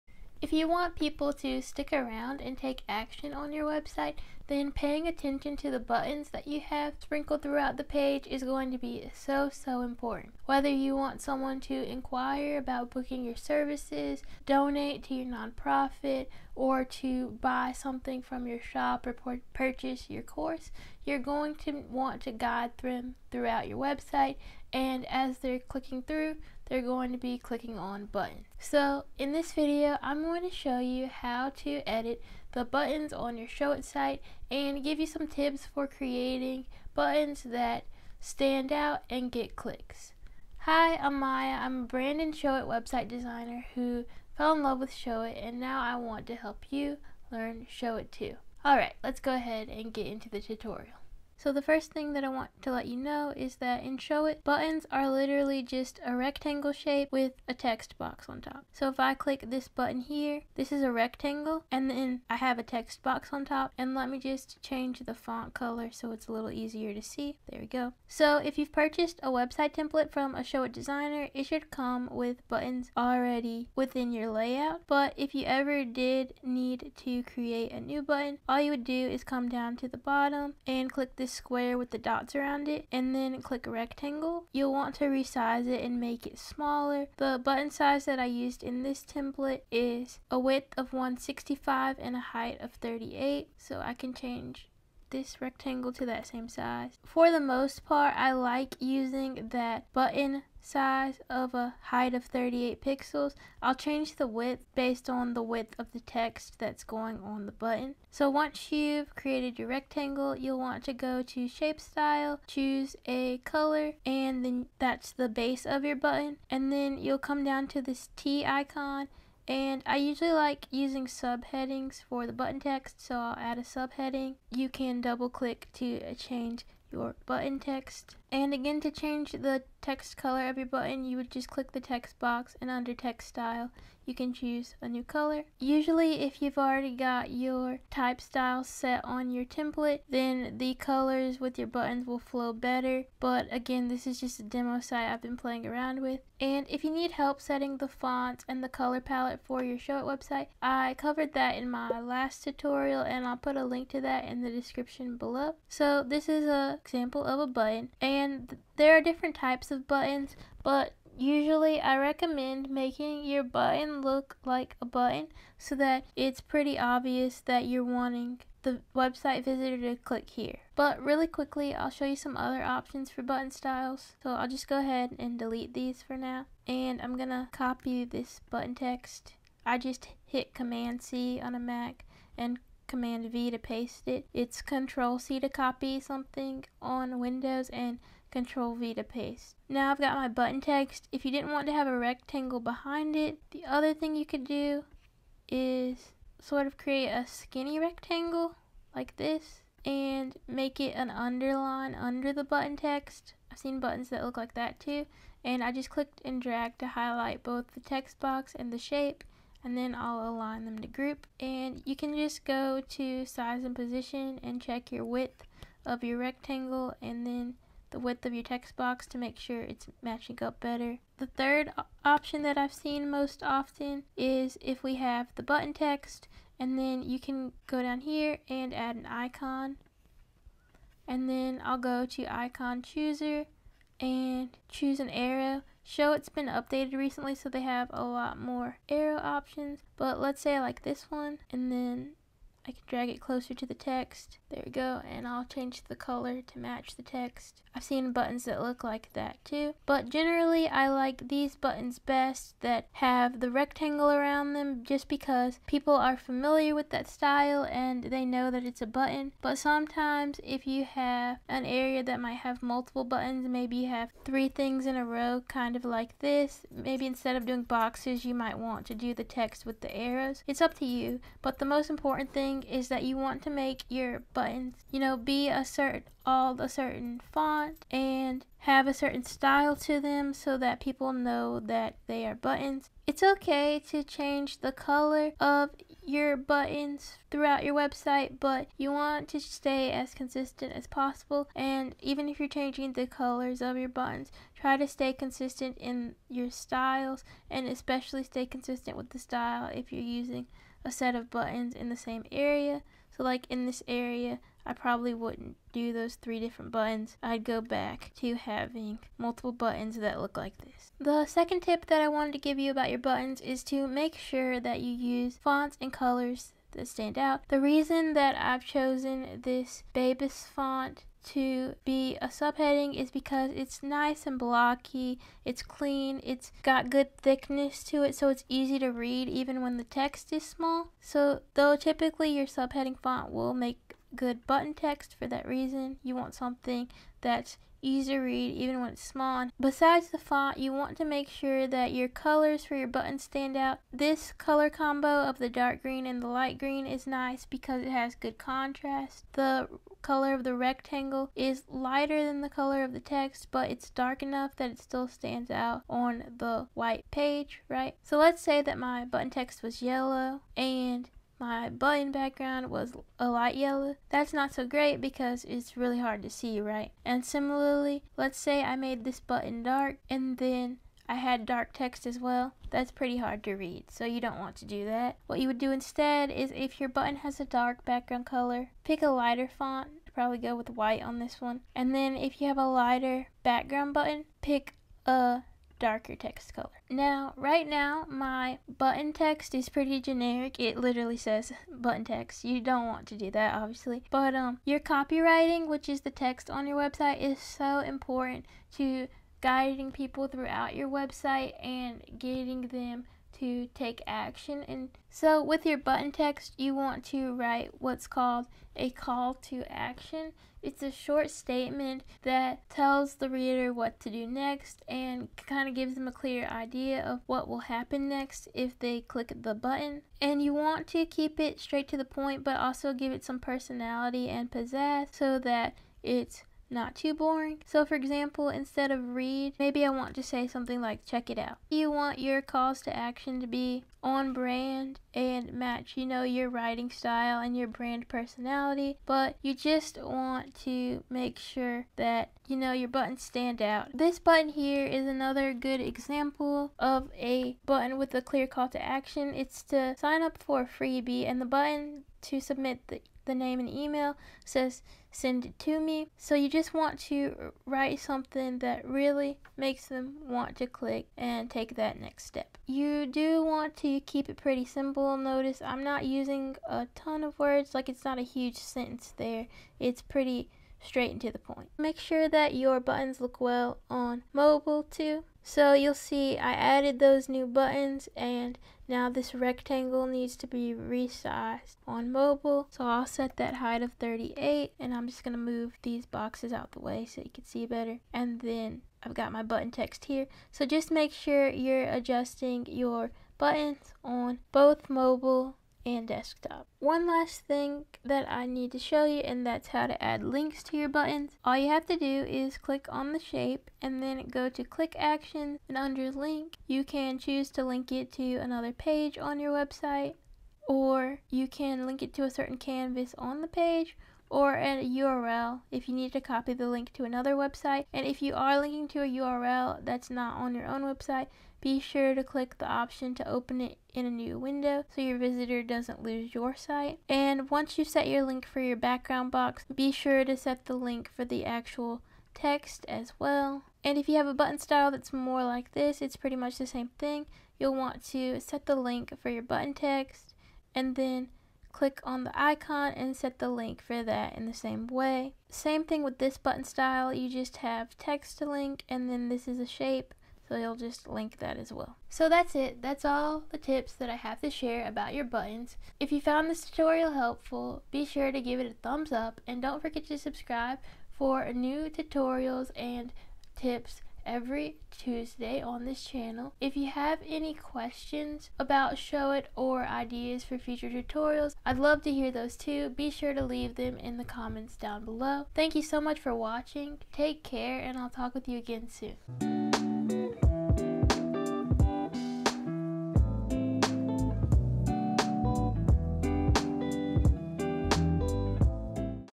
How to Create Buttons for Your Showit Website That Get Clicks
In this Showit tutorial, I'll show you how easy it is to create and edit buttons inside of your website! In this video, I'm also sharing tips for creating buttons that stand out and get clicks. Whether you are starting with a Showit template, or are updating a custom site, this video is for you!

The buttons on your website might seem like a small thing, but they are so important! They play a huge role in getting people to take action on your website.